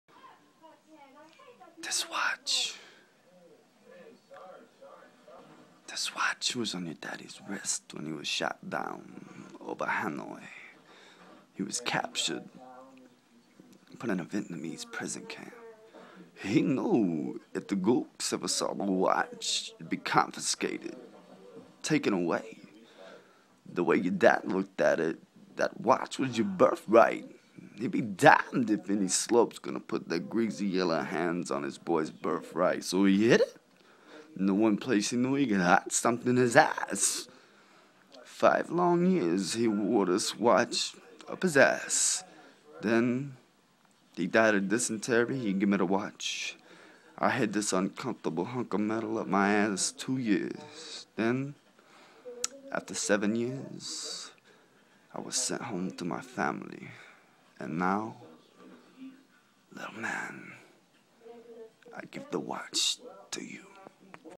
Chris Walken impression!
Me quoting the last of one of my absolute favorite dialogue moments.